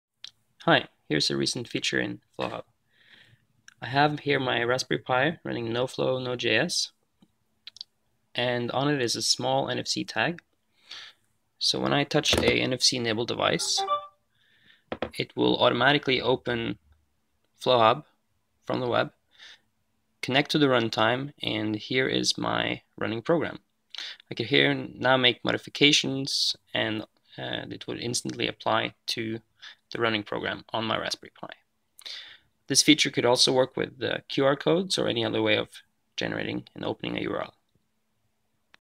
Flowhub live mode open from NFC tag
This feature is out with Flowhub 0.2.0+. We've now released the Kickstarter plans

Note that the Android tablet does not have any special software installed!